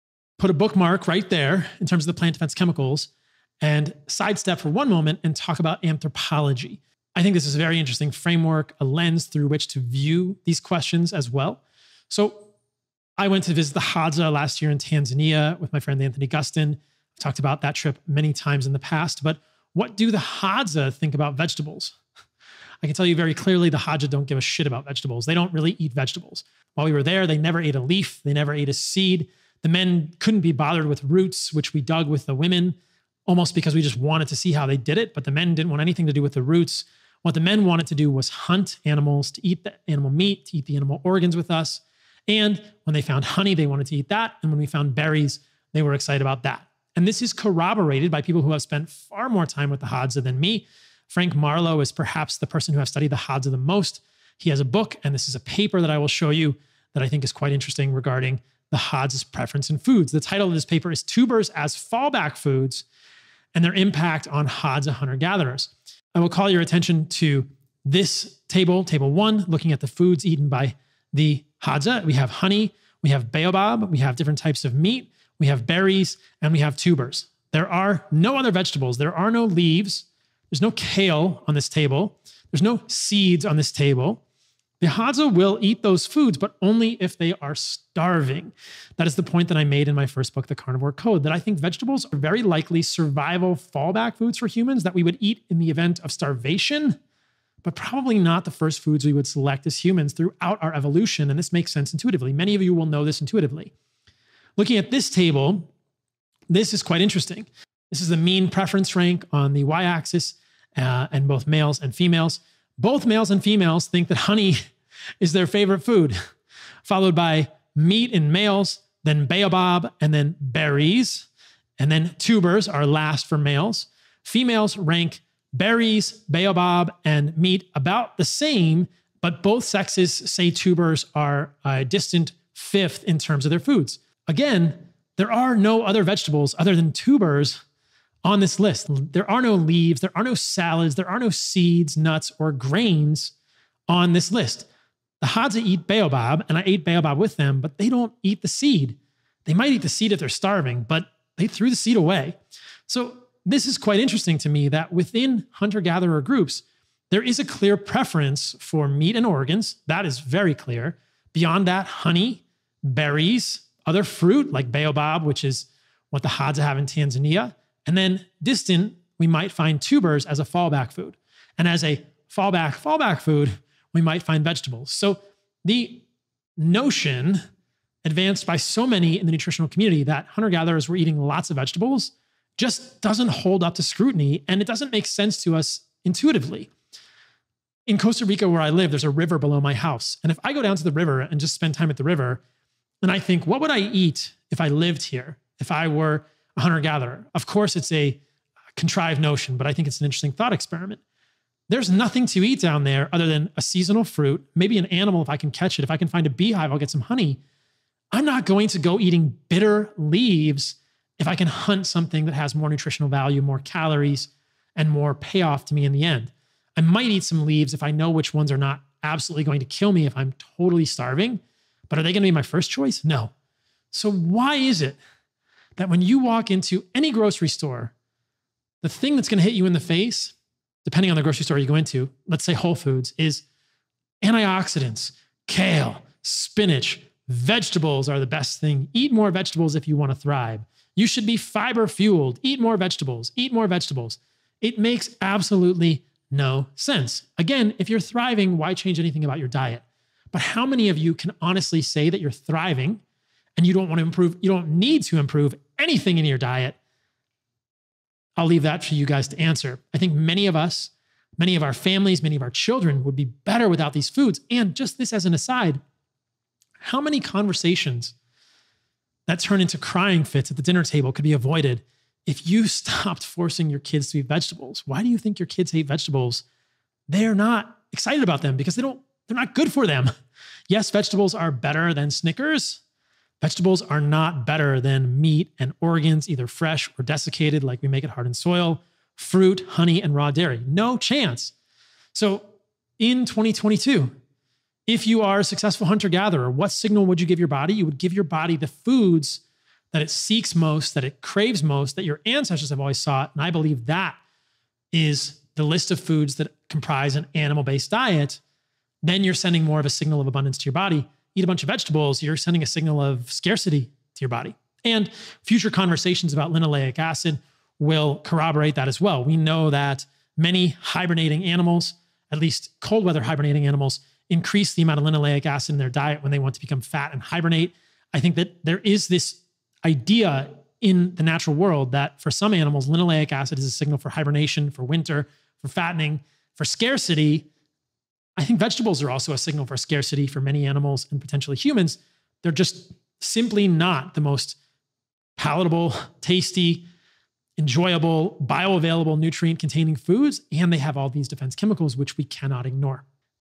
Did our ancestors eat vegetables?
Paul talks about vegetables through an anthropological lens. He uses the Hadza tribe as an example of why meat and organs are the most sought after foods.